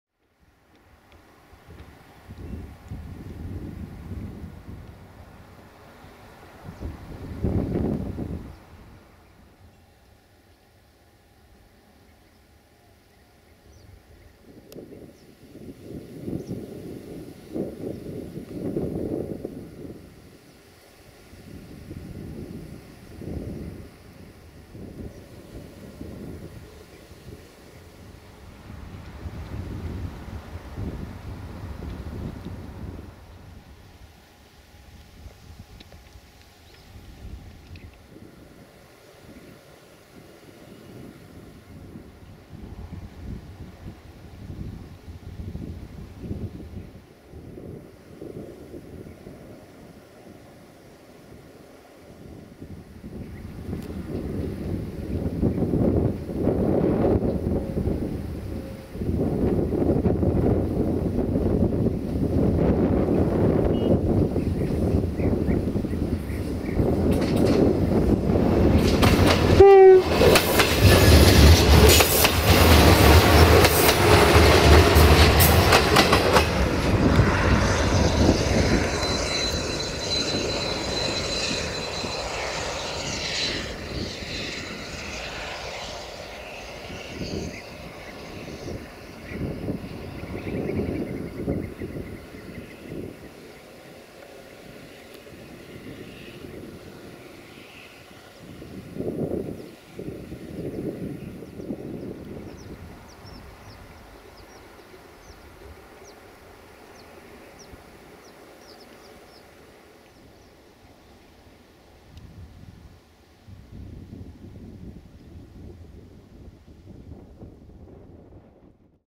IC2 DMU Astra Trans Carpatic IR15582 Mangalia-București Nord in Eforie Nord - 06 September 2019
1:IR1558 - Mangalia - Constanta - Bucuresti Nord - Automotor IC2 MP5721~FP6721 DMU RO-ATC

06 September 2019